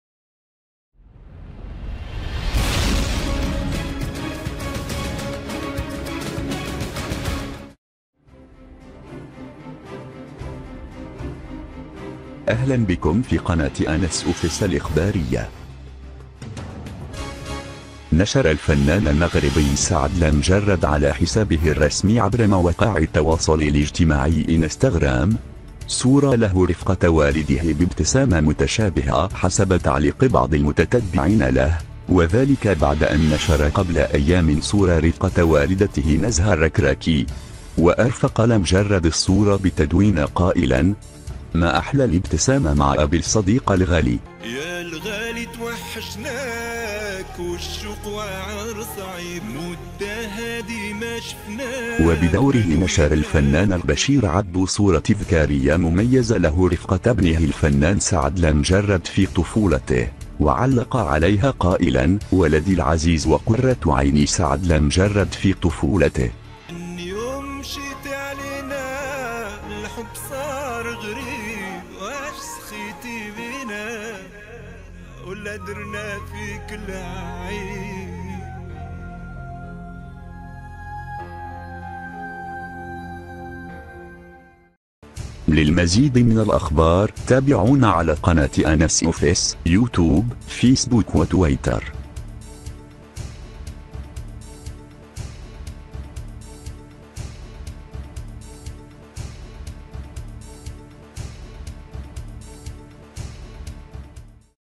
لأول مرة.. سعد المجرد ينشر صورة جديدة رفقة والده البشير عبدو مرفوقة برسالة مؤثرة
نشر الفنان المغربي سعد المجرد على حسابه الرسمي عبر موقع التواصل الاجتماعي إنستغرام، صورة له رفقة والده بابتسامة متشابهة حسب تعليق بعض المتتبعين له وذلك بعد أن نشر قبل أيام صورة رفقة والدته نزهة الركراكي.
وأرفقة لمجرد الصورة بتدوينة قائلا:  "ما أحلى الإبتسامة مع أبي الصديق الغالي".

وبدوره نشر الفنان البشير عبدو صورة  تذكارية مميزة له رفقة ابنه الفنان سعد لمجرد في طفولته، وعلق عليها قائلا؛ "ولدي العزيز وقرة عيني سعد لمجرد في طفولته".

وانهالت التعليقات من طرف متابعي سعد المجرد على الصورتين مؤكدين أن هناك تشابه كبير بين الفنان المغربي ووالد.